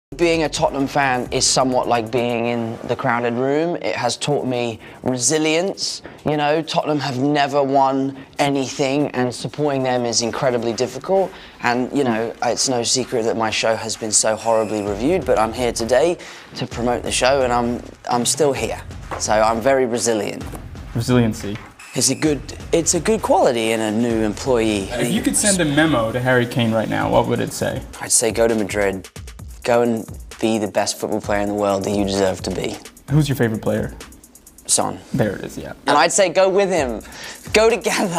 Kane's Precision The Art of Goal Mastery
🚀Experience the extraordinary precision of Harry Kane's goal-scoring prowess Witness the seamless fusion of skill and focus as Kane relentlessly hones in on the net, displaying unmatched aim and scoring finesse.
 From breathtaking long-range strikes to clinical finishes every goal is a testament to his exceptional accuracy Join the journey of a true marksman in the world of football where Harry Kane's goals are not just scores but a symphony of pinpoint precision and unparalleled aim⬇️💯♥️